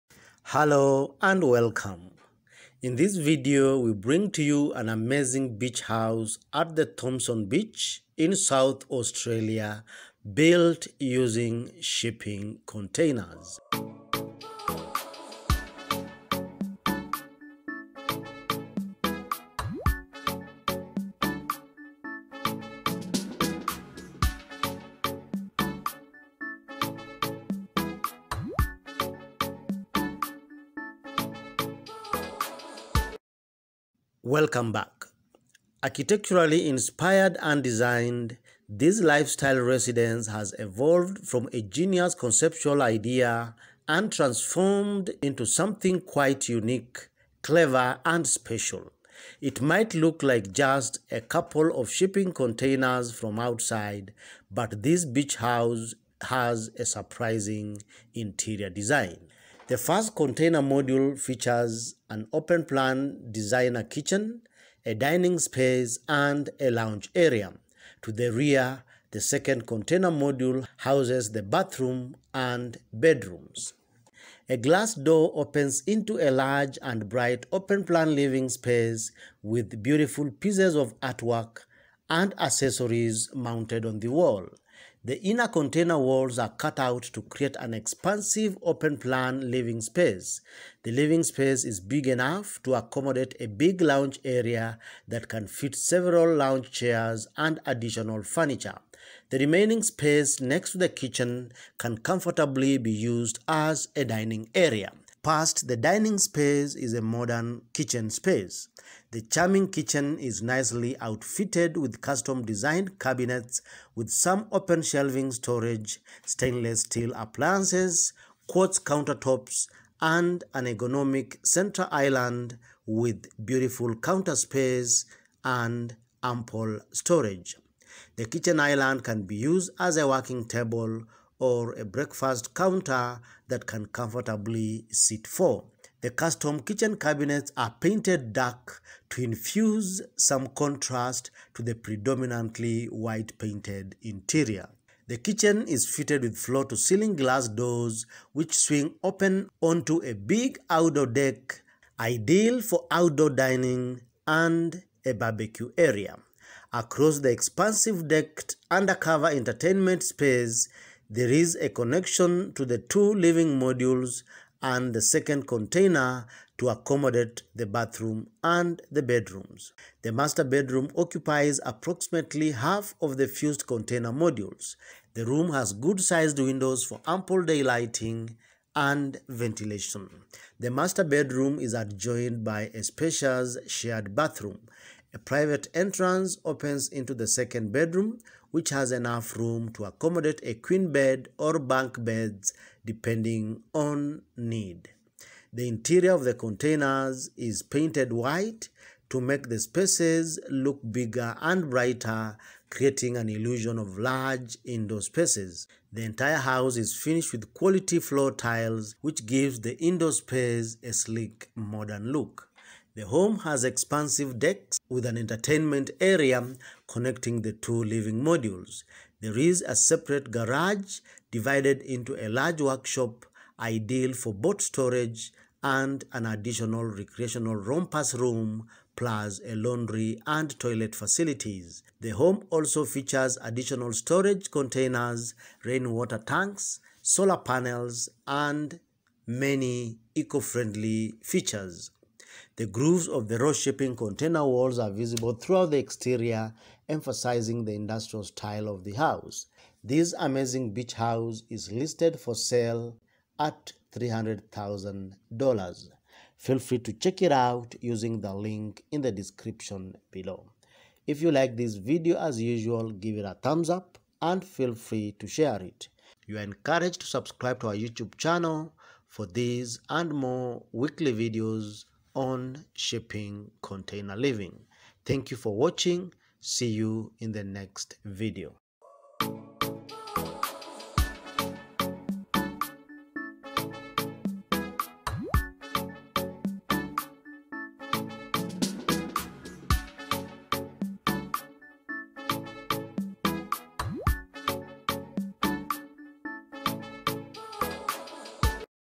Shipping Container Beach House at Thompson Beach, Australia
Shipping Container Beach House at Thompson Beach in South Australia. In this video, we bring you an amazing beach house at Thompson Beach in South Australia built using shipping containers. Architecturally inspired and designed, this lifestyle residence has evolved from a genius conceptual idea and been transformed into something quite unique, clever and special. It might look like just a couple of shipping containers from the outside but this beachside home has a surprising interior. The Thompson Beach home is a two-bedroom residence on a 979sqm block is made from professionally-modified containers and strategically located just a short walk to the beach. The first container module features an open plan designer kitchen, dining and lounge area. To the rear, the second container module houses the bathroom and bedrooms.

Project Summary.
Project: Thompson Beach container home
Architects:
Location: Thompson Beach, South Australia
Containers Used: 4
Property Type: Single Family
Size: 979sqm
Year Built: